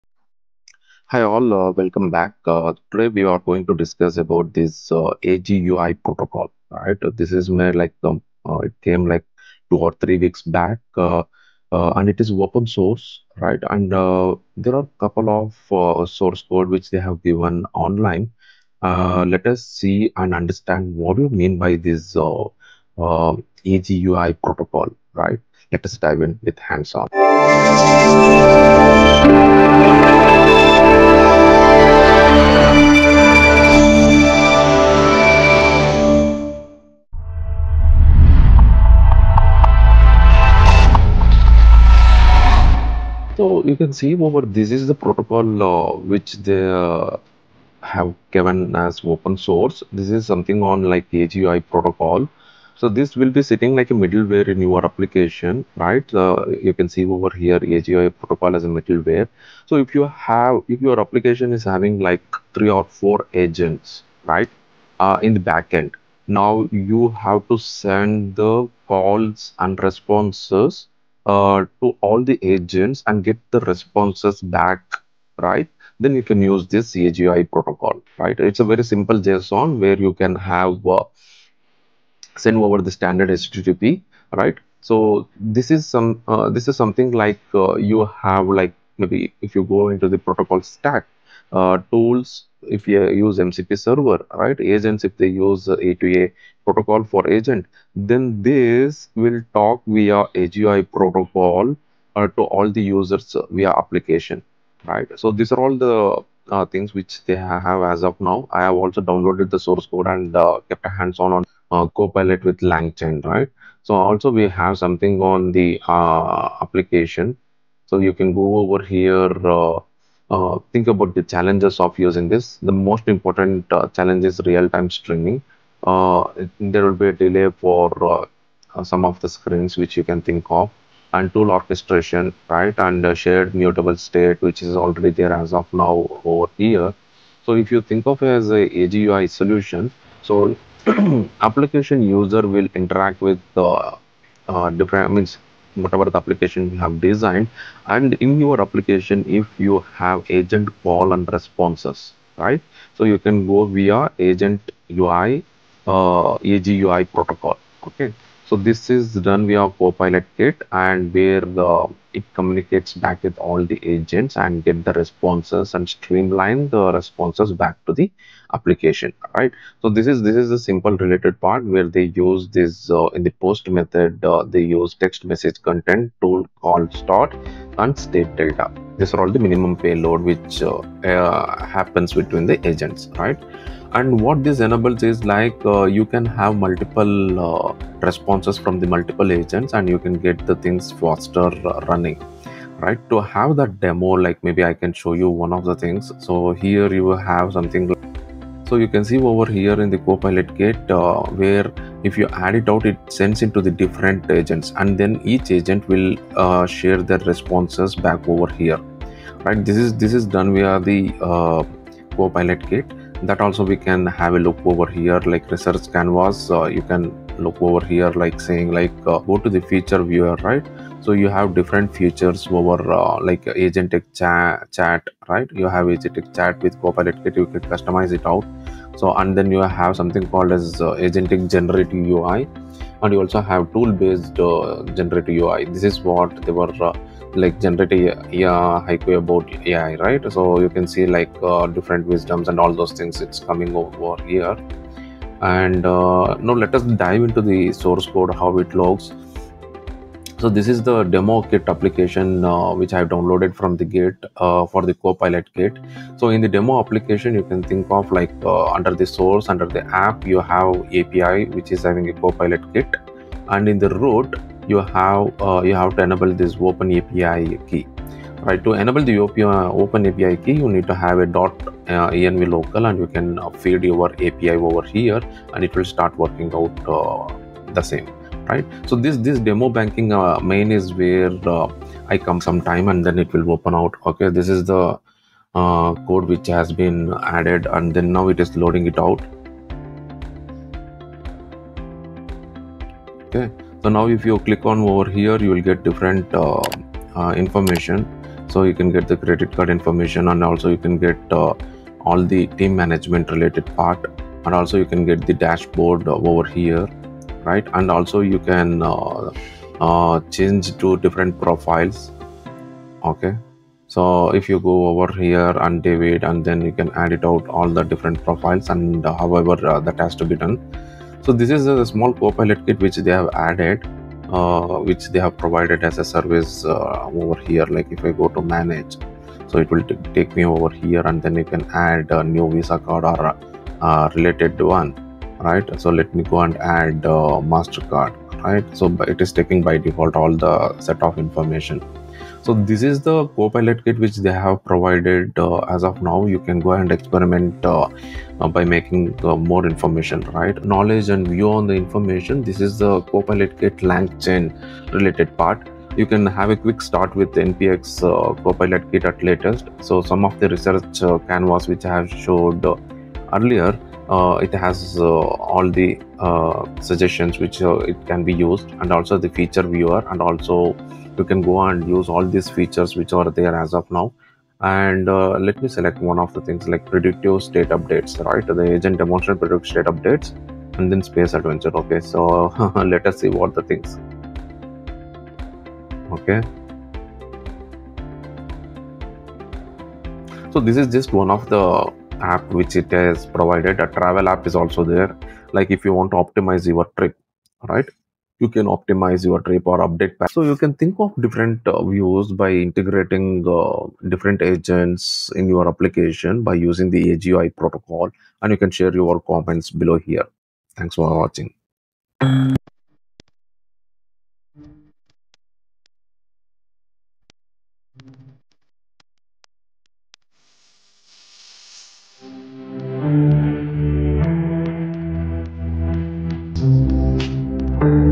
Is AGUI Protocol The RIGHT Choice For Your Agent Integration Needs?
AGUI Protocol typically stands for Agent-based Graphical User Interface Protocol. It is used to describe the interaction mechanism between autonomous agents (AI systems) and a graphical user interface layer, allowing agents to control, interpret, or simulate UI behavior.

🔸 Example Use Cases
AI-powered testing: AGUI protocol allows bots to run through UI test cases like a human would.

Agentic UI automation: LLMs use the protocol to simulate real-time interaction with apps or websites.

Low-code RPA (Robotic Process Automation): Automates clicking, input, and navigation.